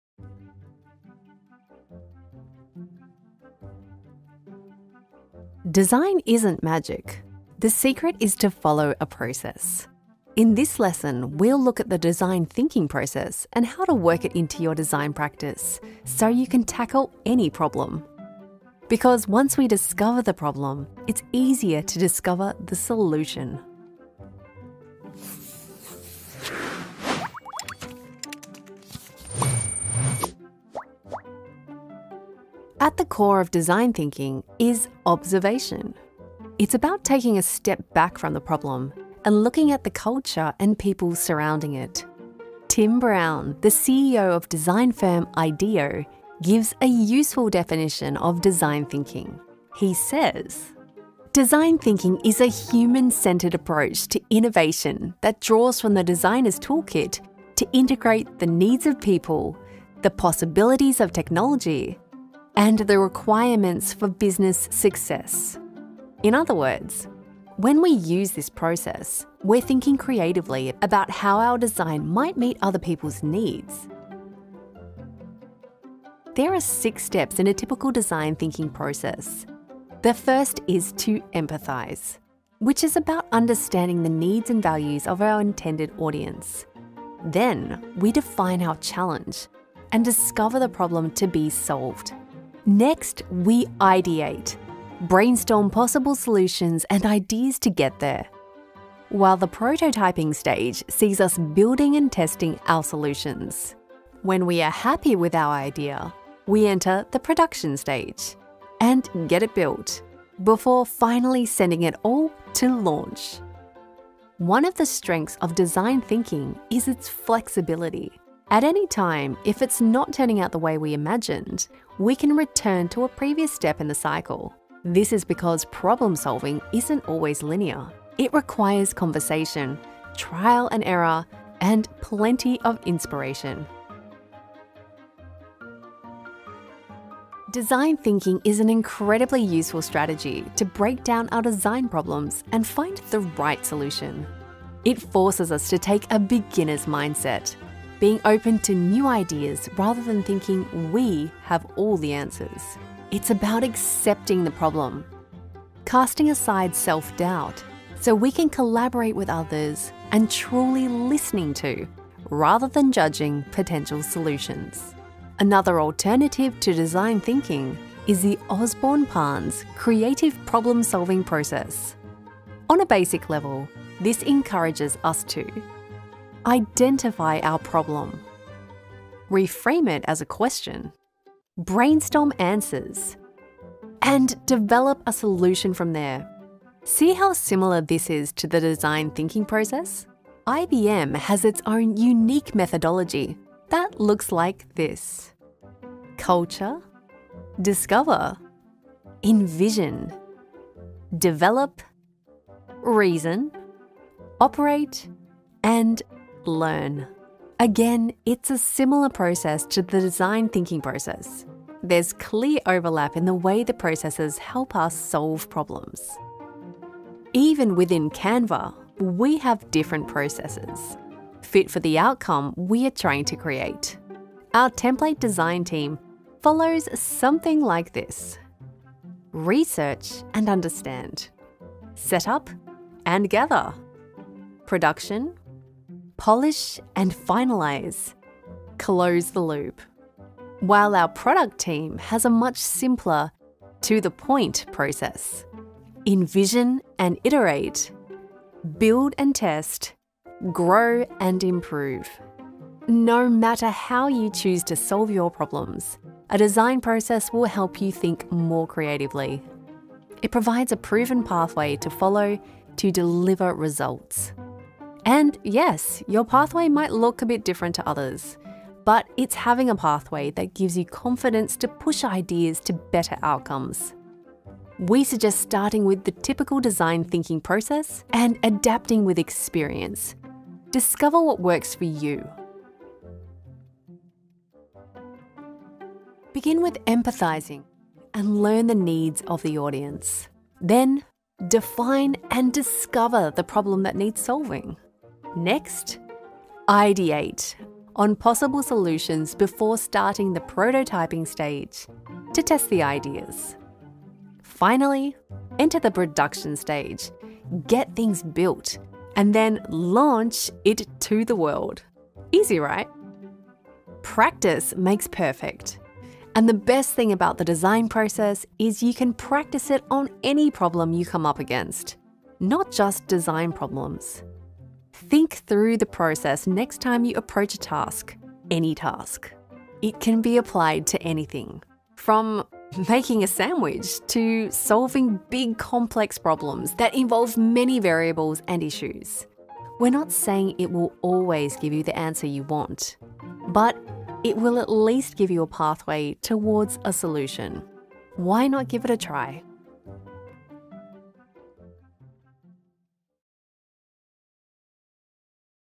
1. Introduction to the Design Process | Theory
Discover what design thinking is and how design thinking works in real life.
Design is not magic and designers aren't wizards. We follow a process that helps us understand problems. Once you know the problem it's a lot easier to see the solution. This video serves as a great starting point to understand the design process without going too deep into what each step contains.

► What is Design thinking
► The 6 steps to solving Design problems
► How Design thinking work
► How to finding a design process that works for you
► What are some popular Design processes

✏️  CANVA GRAPHIC DESIGN COURSE
This video is part of our Graphic Design Course. The course is made out of Theory lessons and Skill ones, where we take you to the Canva Editor to design!
This video is part of Unit 02, make sure you watch the rest of the course:

😎 IN THE MOOD FOR MORE VIDEOS LIKE THIS ONE?